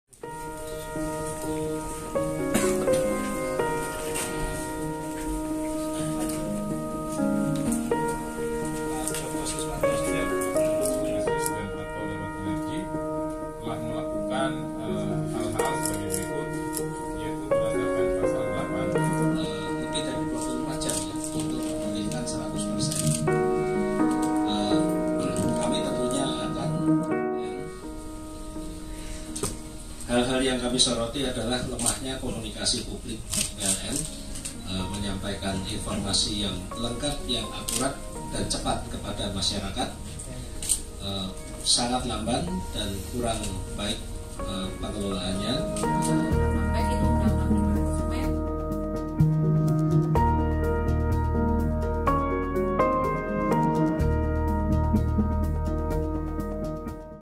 Ombudsman Kritik Lemahnya Komunikasi PLN ke Masyarakat Saat Listrik Padam
MERDEKA.COM - Ombudsman soroti sikap PLN atas kejadian mati listrik total, blackout, Minggu (4/8). Anggota Ombudsman, PLN lemah menyampaikan informasi ke publik selama listrik padam di Pulau Jawa. Informasi yang disampaikan perusahaan listrik negara itu dinilai sangat lamban dan tidak baik pengelolaannya.